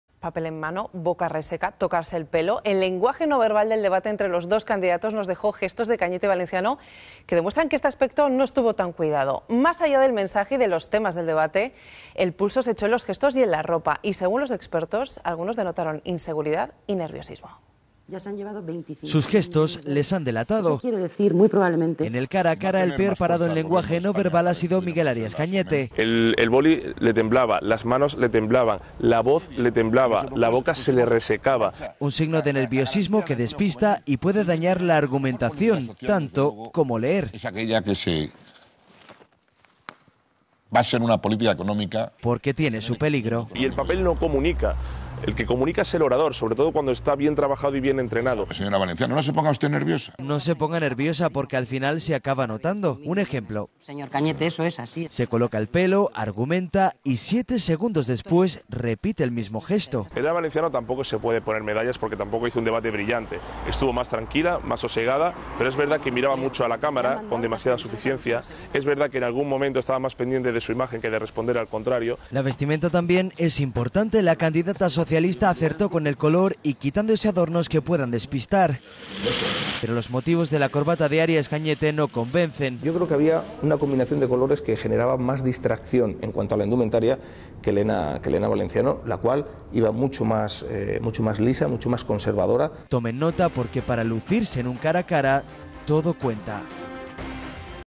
El lenguaje no verbal deja en evidencia a Cañete y Valenciano durante el cara a cara
Papel en mano, boca reseca, tocarse el pelo... el lenguaje no verbal del debate entre los dos candidatos nos dejó gestos de Cañete y Valenciano que demuestran que ese aspecto no estuvo tan cuidado. Más allá del mensaje y de los temas de debate, el pulso se echó en los gestos y en la ropa. Y, según los expertos, algunos denotaron inseguridad en Valenciano y nerviosismo en Cañete.